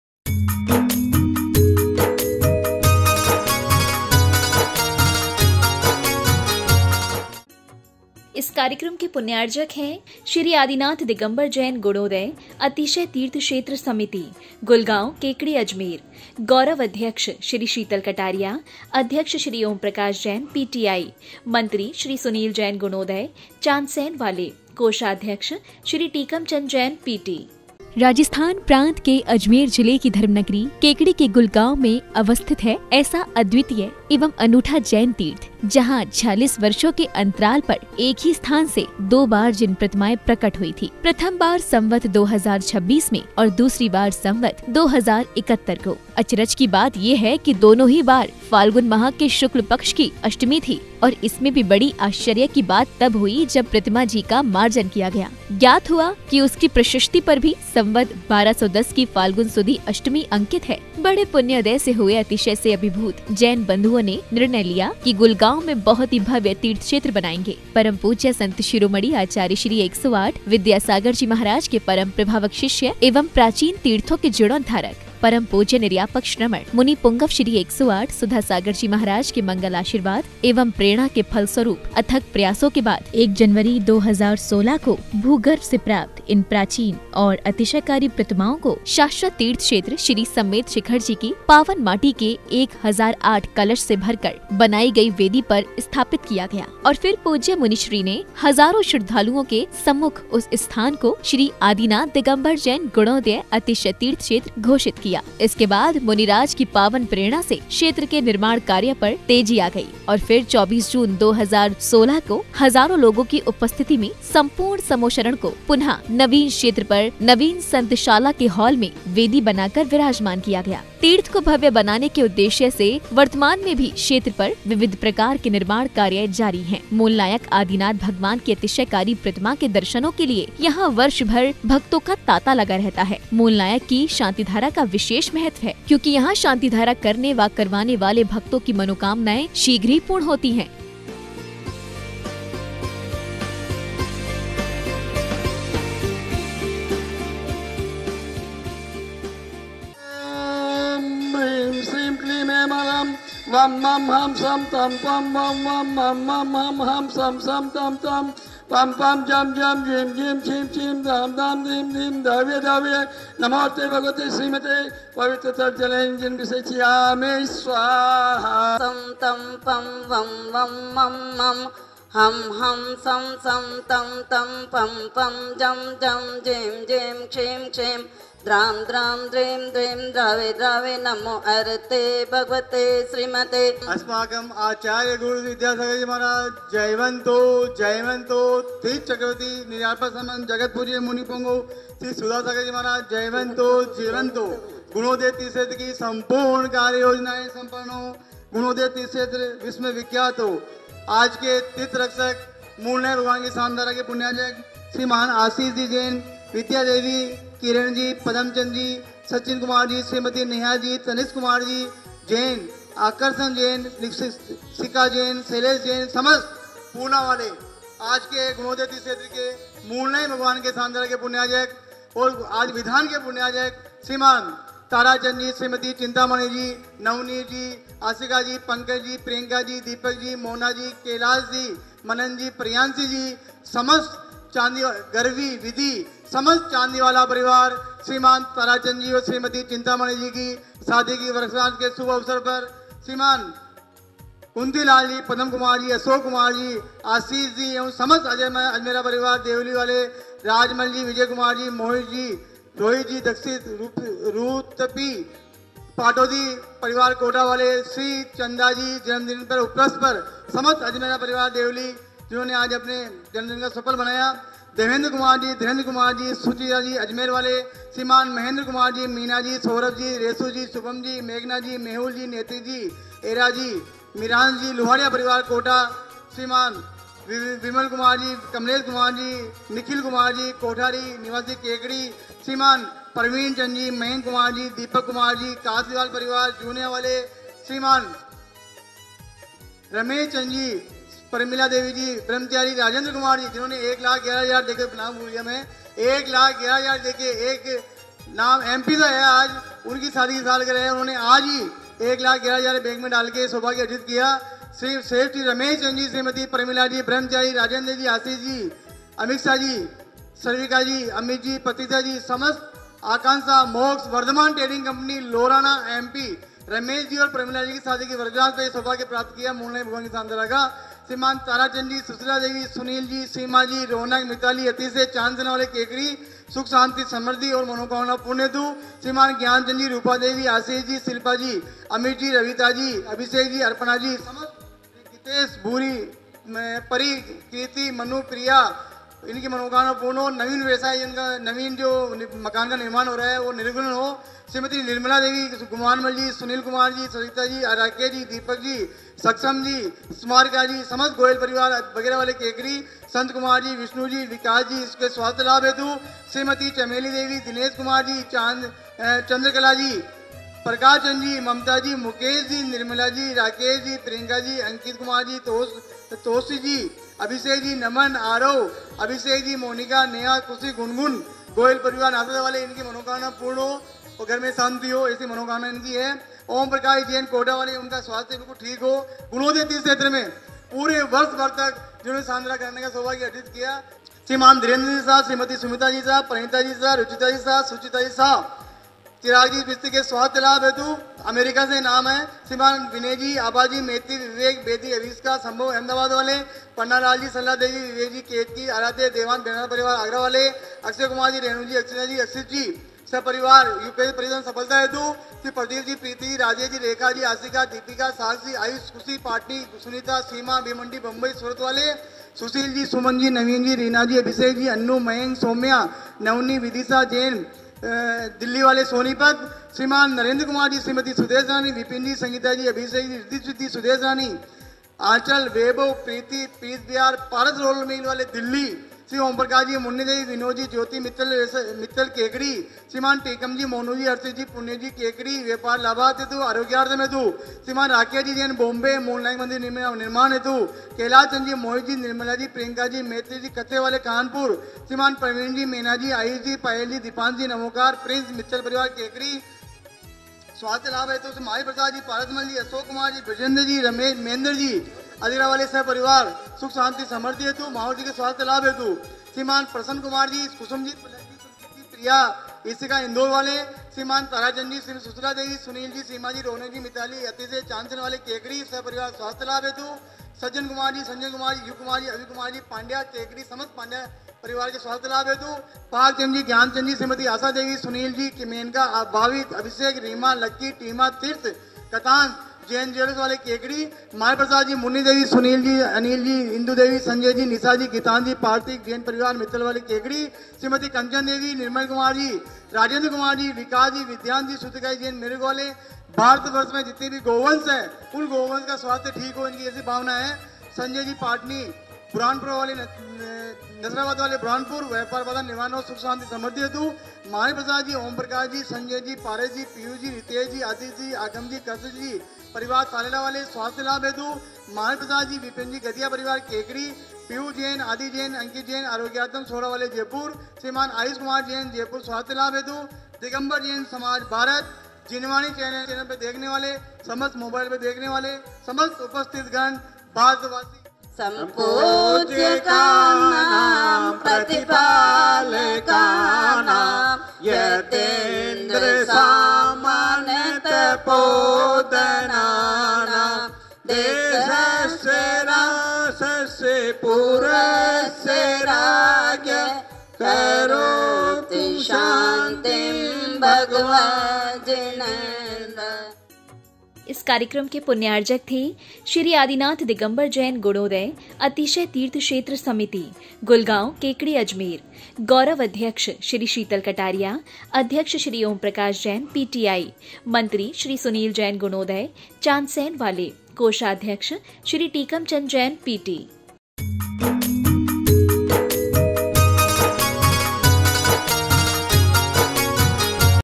श्री आदिनाथ भगवान | अभिषेक एवं शांतिधारा | Gulgaon, Kekri | Ajmer | 14 May 2024 | A011768
मूलनायक 1008 श्री आदिनाथ भगवान की प्राचीन एवं अतिशयकारी प्रतिमा का अभिषेक एवं शांतिधारा
स्थान- श्री आदिनाथ दिगम्बर जैन गुणोदय अतिशय तीर्थक्षेत्र गुलगांव, केकड़ी, जिला-अजमेर (राज.)

E A011768 SHANTIDHARA GULGAON KEKARI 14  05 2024
Jinvani Channel Apps :